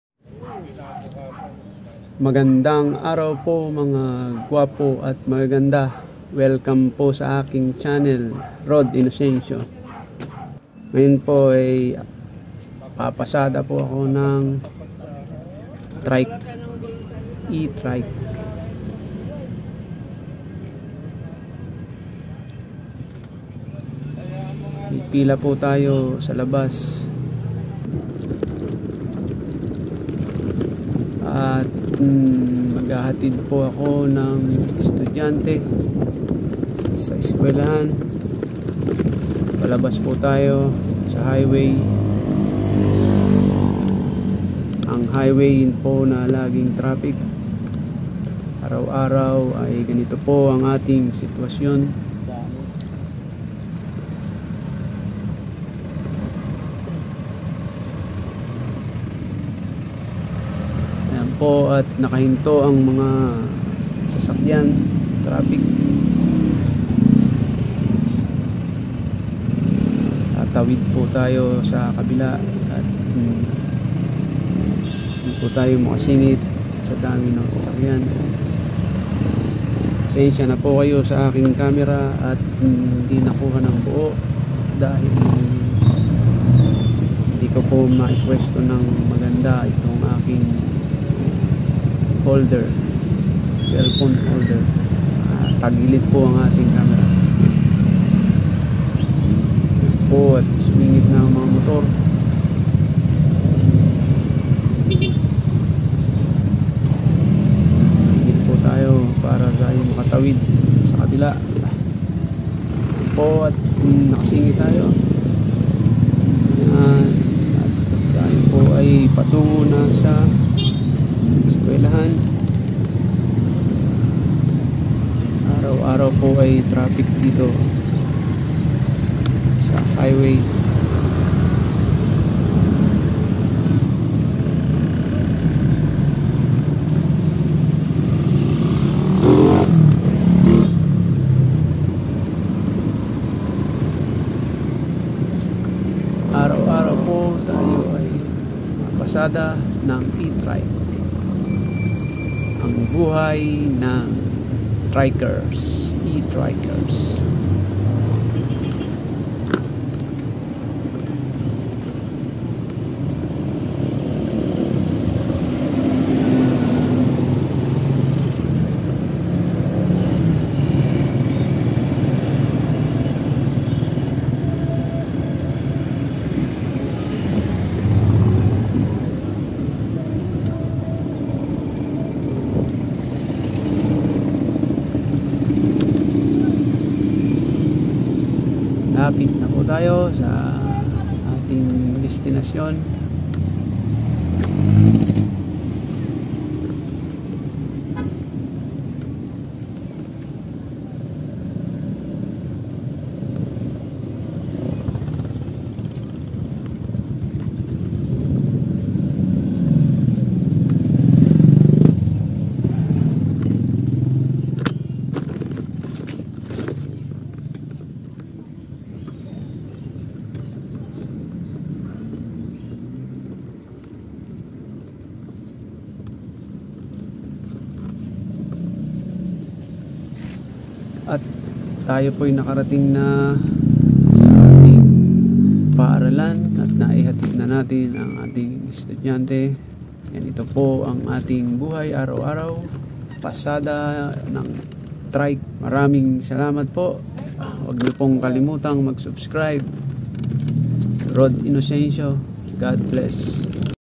E - TRIKE PASADA
papasada po ako ng e trike. maghapon sa initan at usok ng sasakyan. sakay na po kayo.